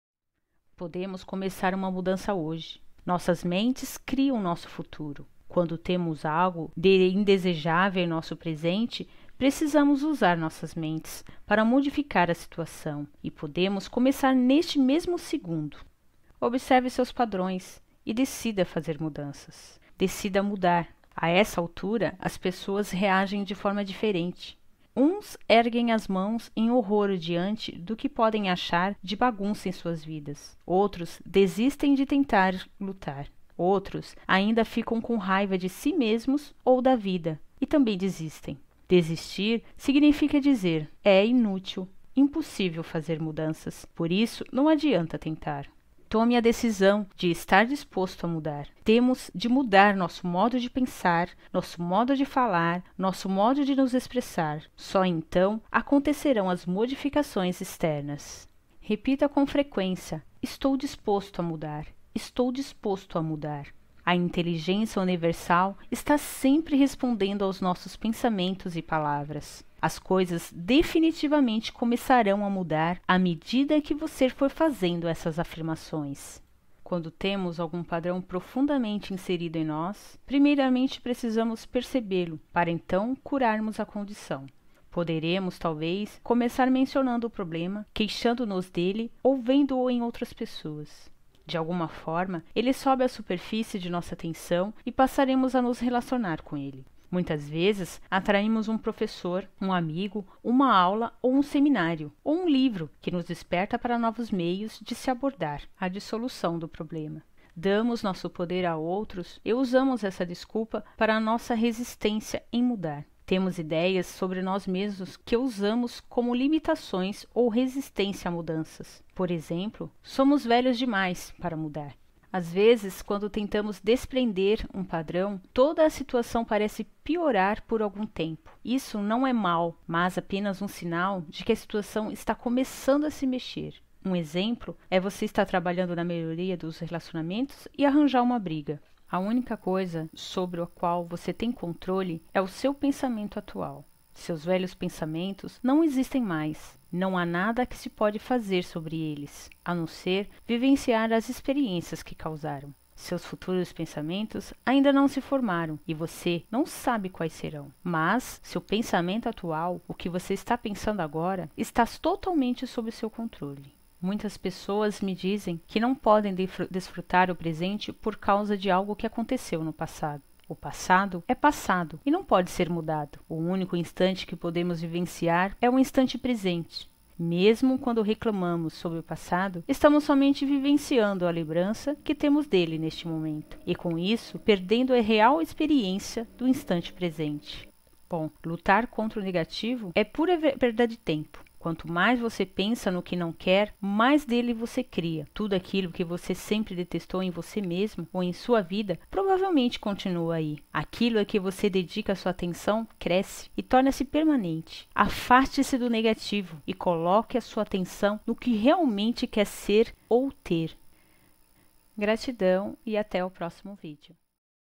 Louise L. Hay - Mude a sua Vida!
A força de cada pessoa vem de dentro. Assim, para ir mais longe, crescer e fortalecer, é preciso se descobrir e conhecer a si mesmo. E ela chama a atenção para as vantagens do pensamento positivo.

Livro: Você pode curar sua vida - Autora Louise L. Hay

Gratidão!